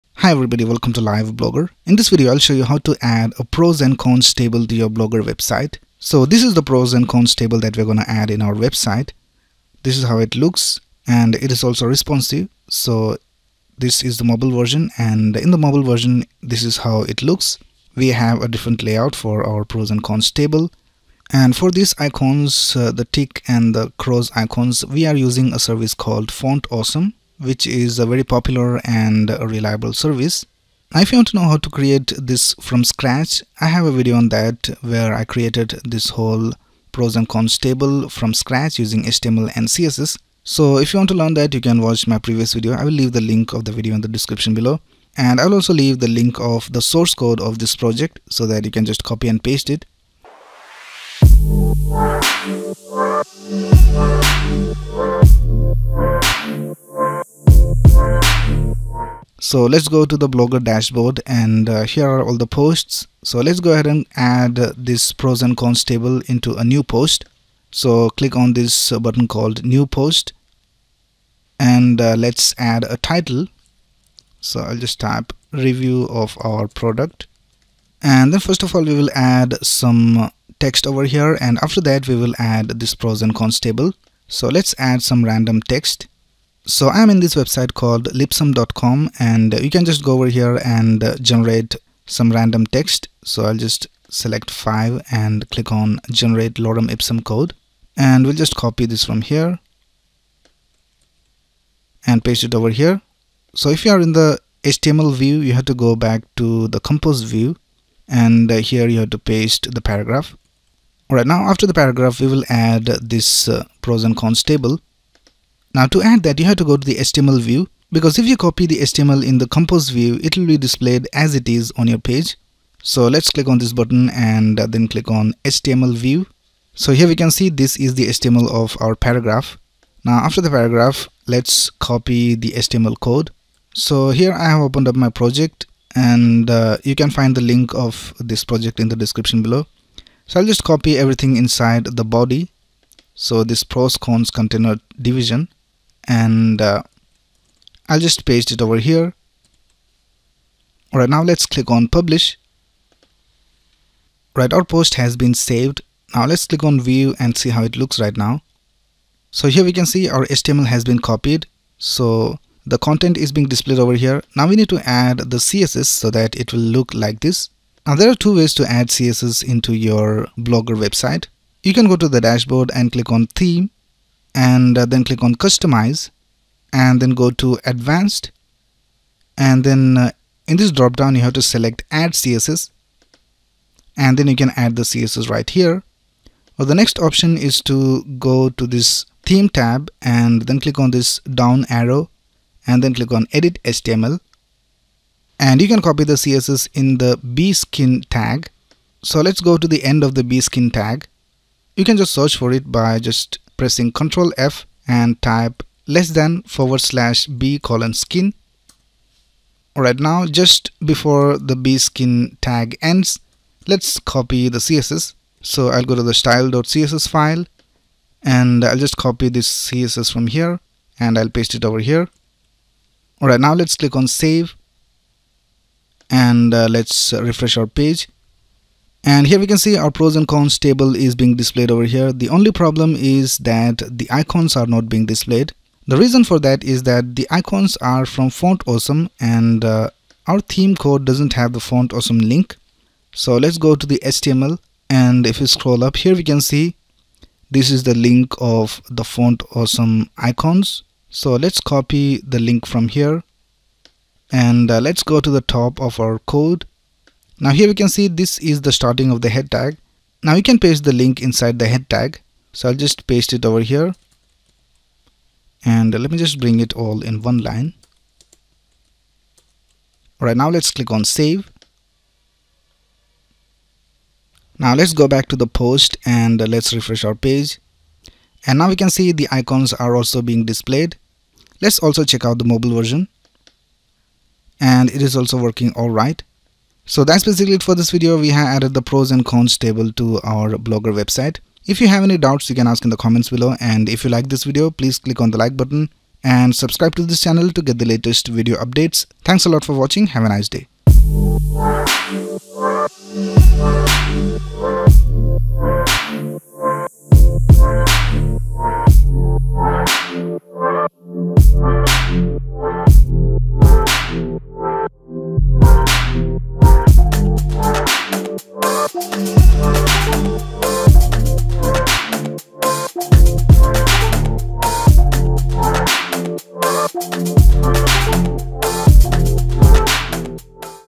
How To Add Responsive Pros And Cons Table To Your Blogger Website
Hi guys, in this video, I will show you how to add a Pros and Cons Table to your Blogger website. It will be responsive as well. I hope you find this useful.